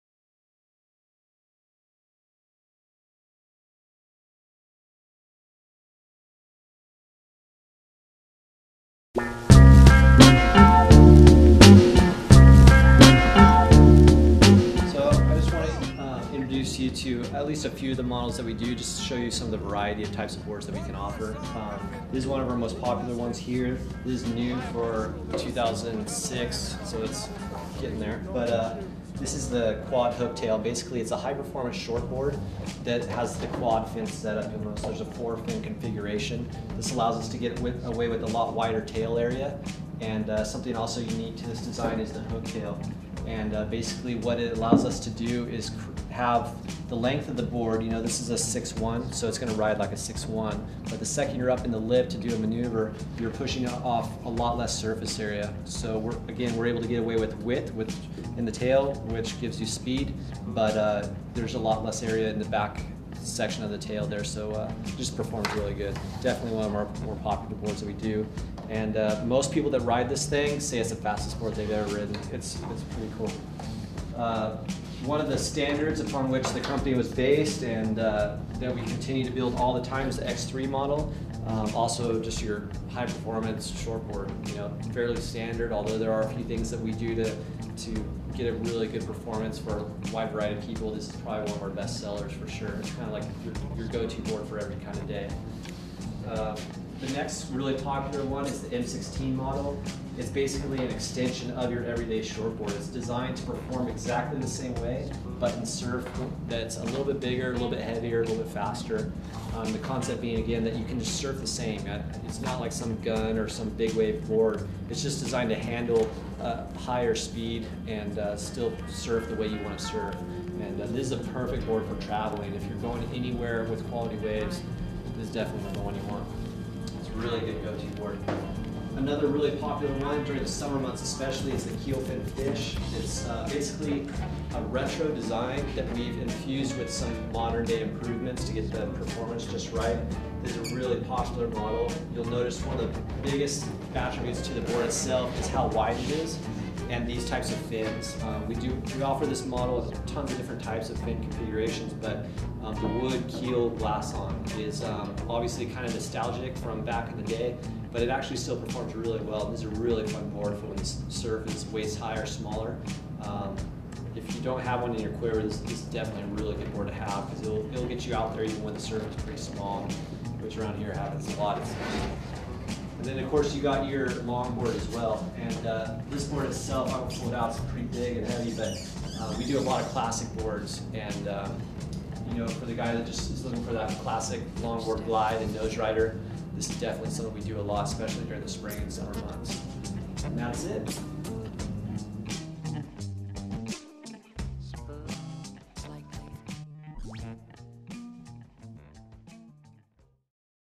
Walker Project Surfboard Lineup
Mike Walker gives you a look at the selection of surfboards offered by his company, The Walker Project. Whatever type of surfer you are, Mike's got a board for you.